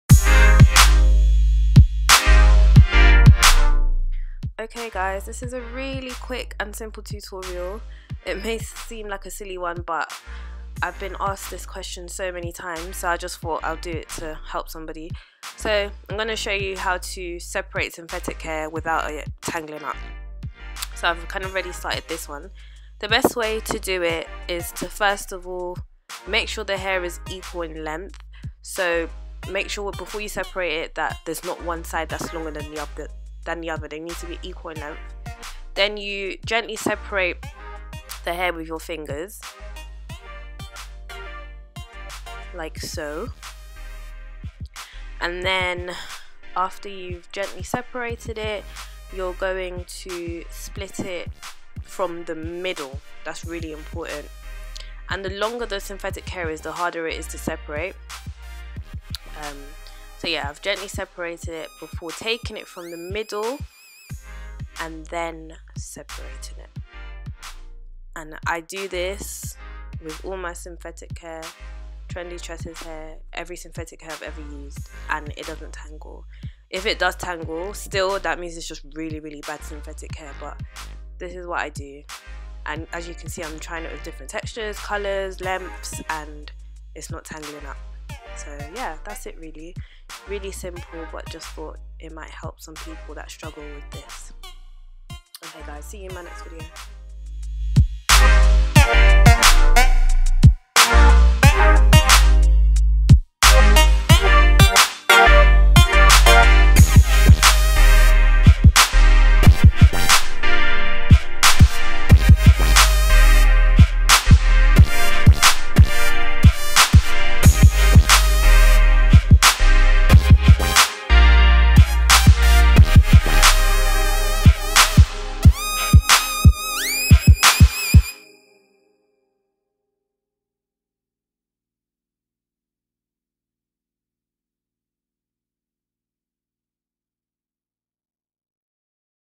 How to separate synthetic hair NO TANGLING! l FT Trendy Tresses
Hey guys!

Highly requested! I know synthetic hair can be a nightmare to detangle and separate so thought i'd make  quick tutorial on how to do just that with no tangling!